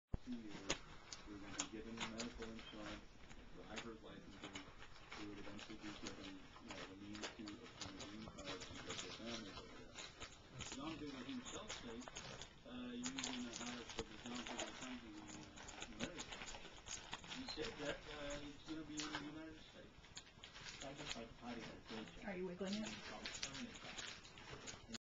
The Toe Nail
Someone's very scary, very sculpted toe nail.